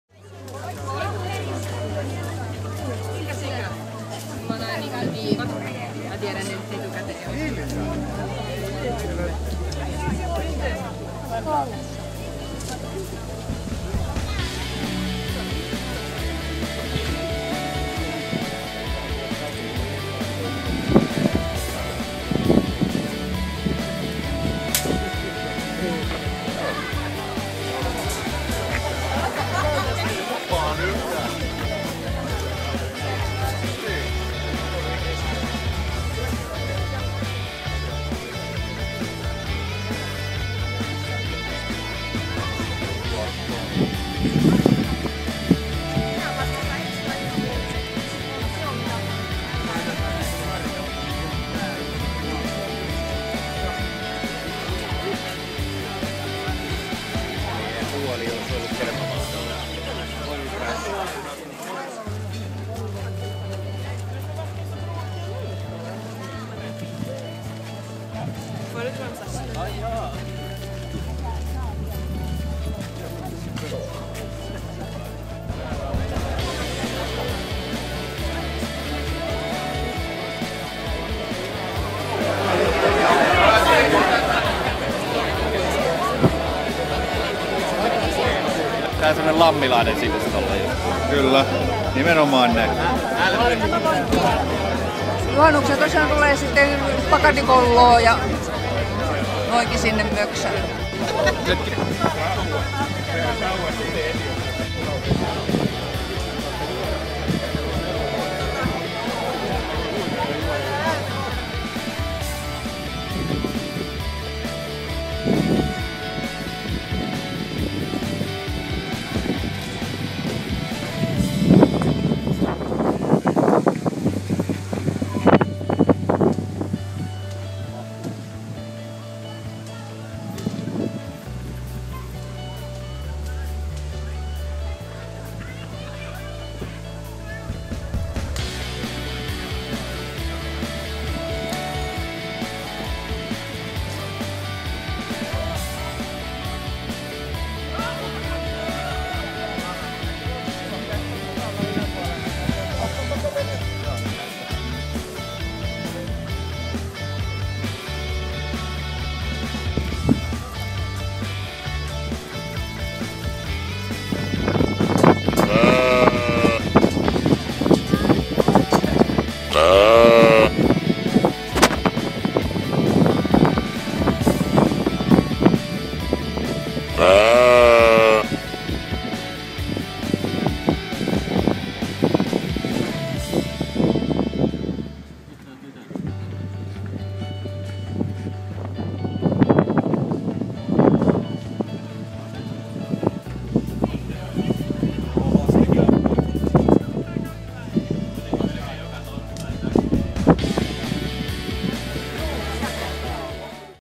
Lammin Pellavamarkkinat 2013
Tunnelmapaloja perinteisiltä ja legendaarisilta Lammin Pellawa- ja sahtimarkkinoilta 2013.